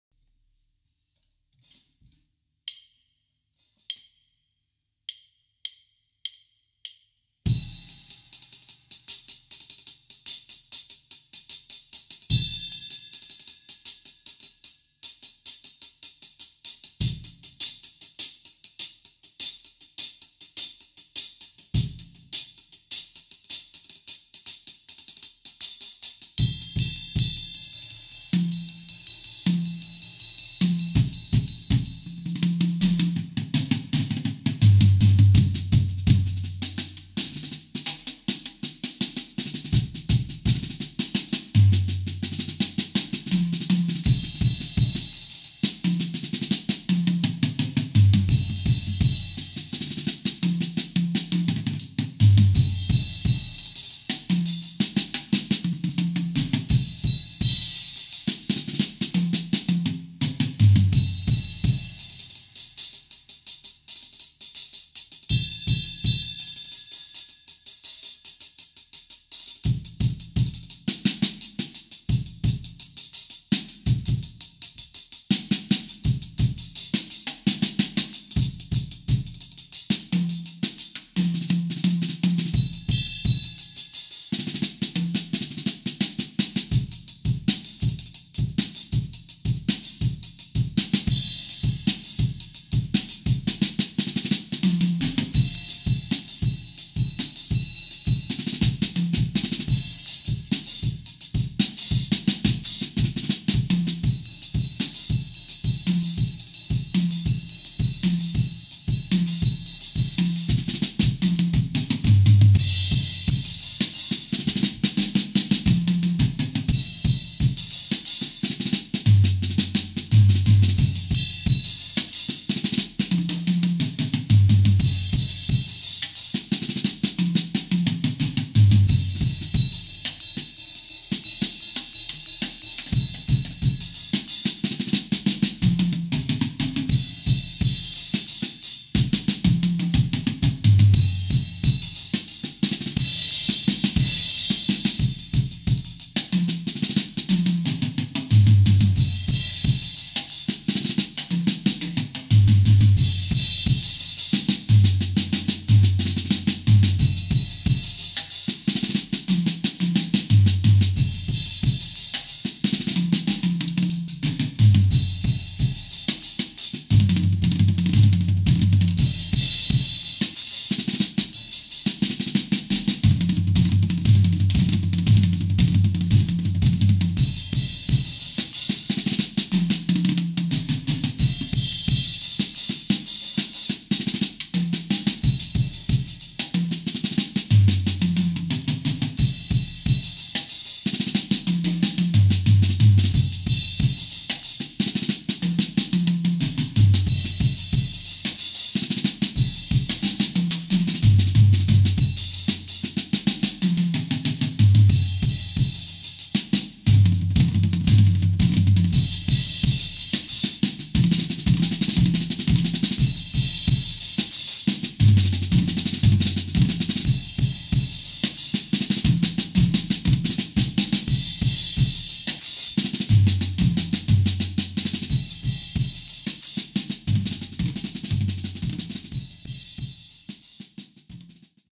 My Test 1
My First Test Playing after 14 Years In ACZ Emmen